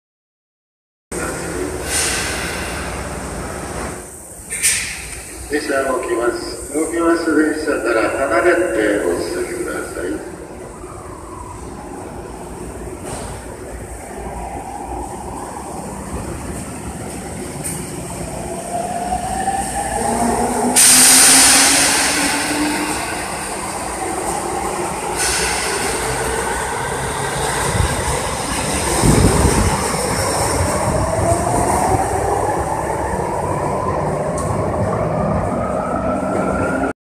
都営新宿線 10-409F(8両編成) 各駅停車 新線新宿行き 九段下にて撮影
都営新宿線 各駅停車 新線新宿行きです。終点まで各駅に停まります。九段下にて撮影しました。